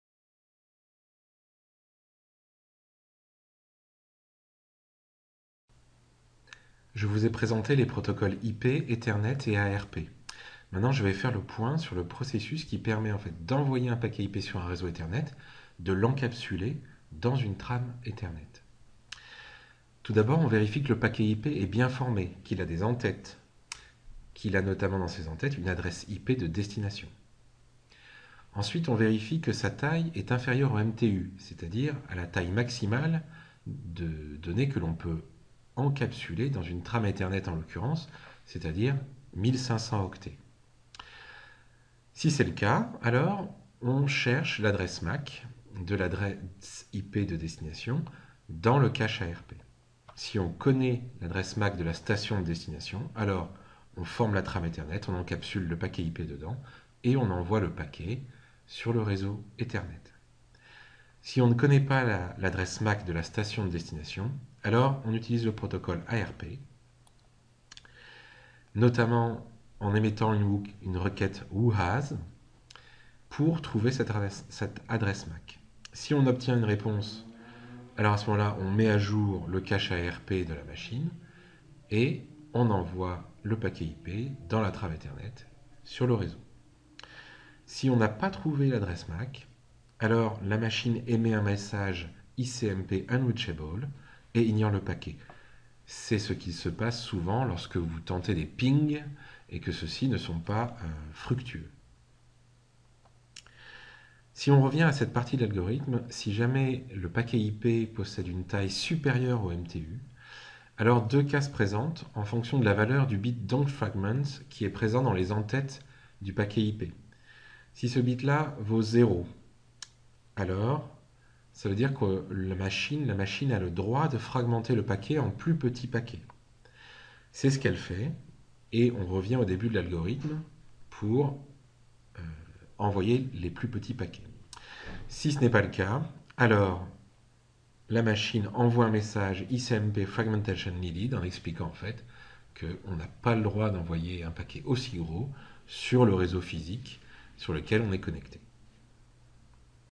Comment une station émet-elle un paquet IPv4 sur un réseau Ethernet ?
les meilleurs moments de mon cours... en vidéo.